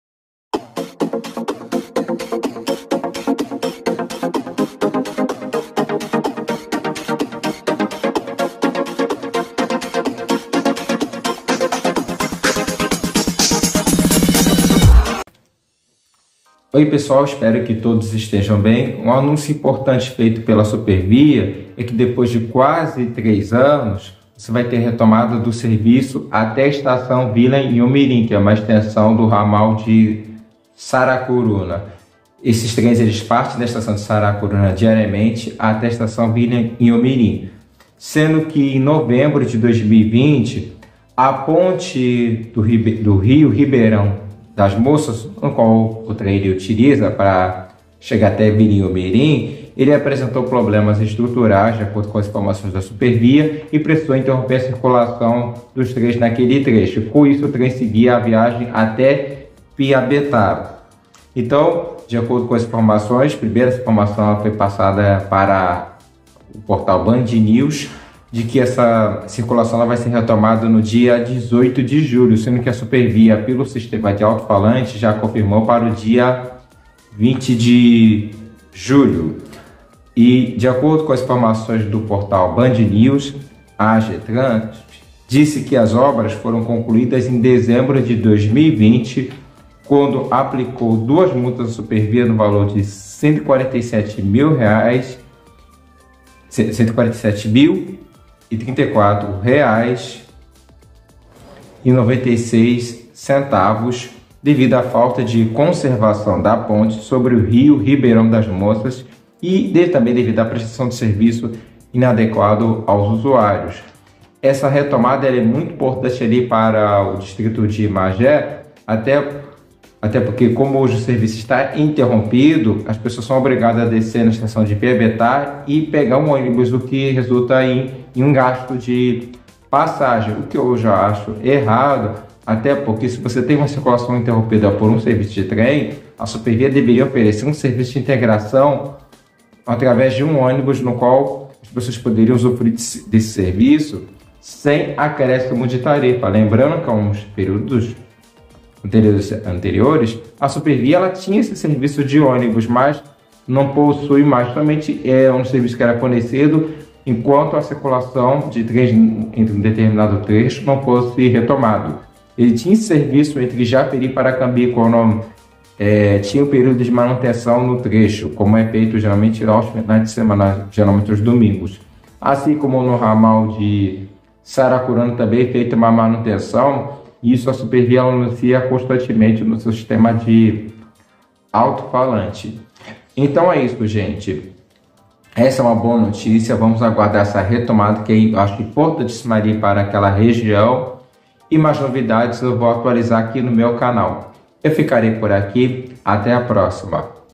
Supervia anuncia a volta da circulação dos trens até Vila Inhomirim
A circulação havia sido interrompida desde novembro de 2020, quando apresentou problemas estruturais na ponte ferroviara que passa sob o Rio Ribeirão das Moças.